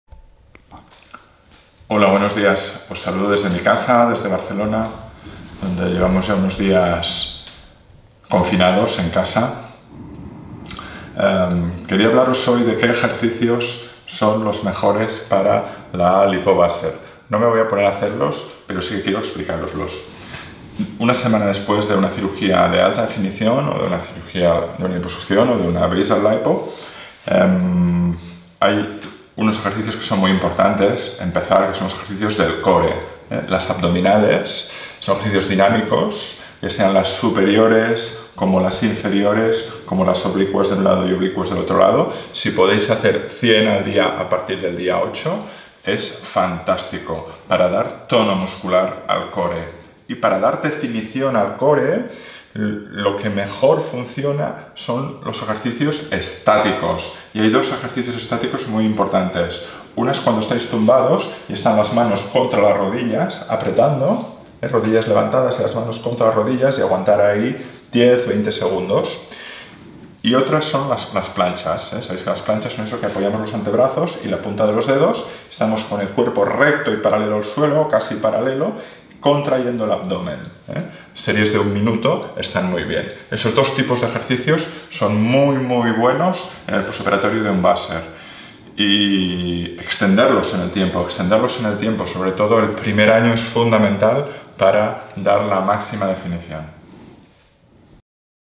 Ejercicios Core
Ejercicios Core para un posoperatorio de una lipo Vaser de alta definición que se pueden practicar des de casa.